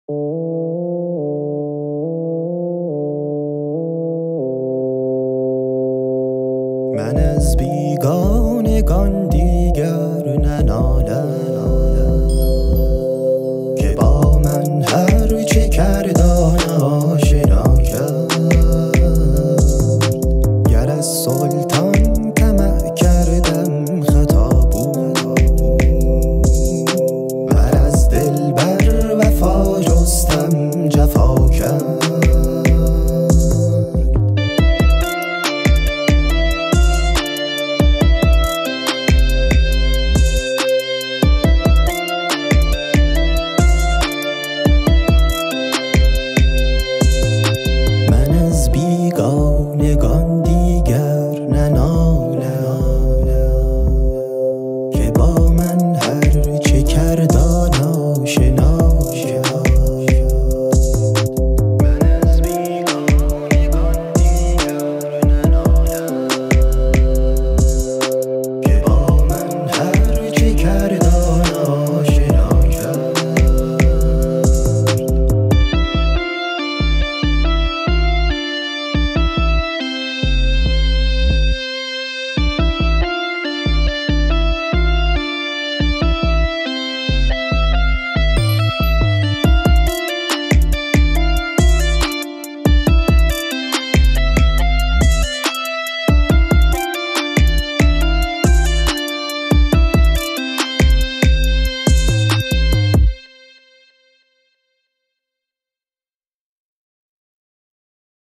Melodilerle Aruz (2) - Hezec-i Müseddes-i Mahzûf
Bahir: Hezec-i Müseddes-i Mahzûf
Vezin: Mefâîlün Mefâîlün Feûlün
Beyit: Hâfız-ı Şîrâzî
Müzik, Mix, Master: kadirdemir

Beyitler:

من از بیگانگان دیگر ننالم
که با من هر چه کرد آن آشنا کرد

گر از سلطان طمع کردم خطا بود
ور از دلبر وفا جستم جفا کرد

Anlamı:

Ben artık yabancılardan şikayet etmiyorum
Bana her ne yaptıysa tanıdıklar yaptı

Sultandan bir şey beklediysem hataydı
Sevgiliden vefa beklediysem de cefa etti